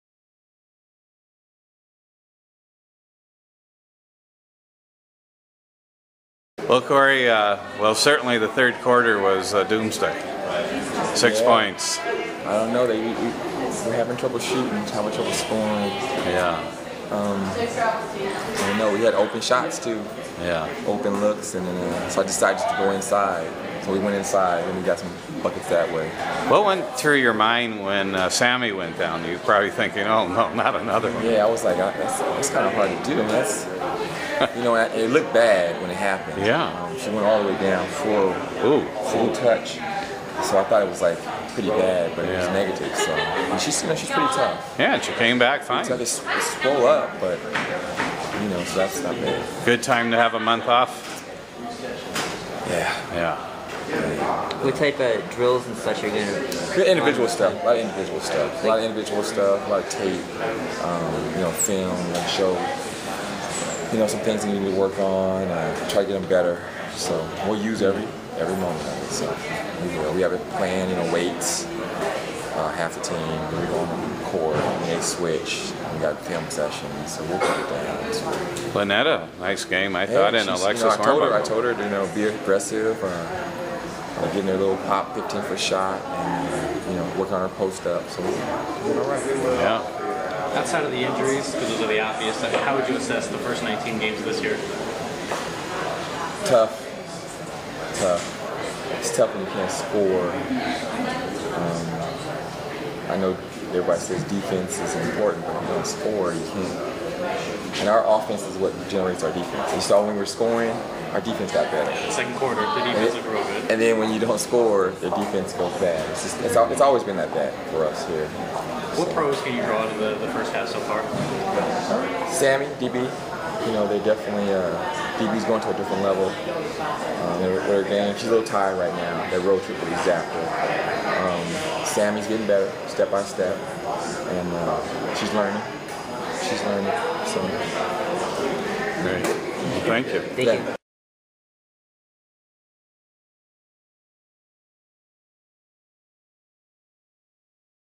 July 13, 2012 Corey Gaines Mercury vs Storm
7/13/12 Post Game Interview with Coach of the Phoenix Mercury (Seatle Storm)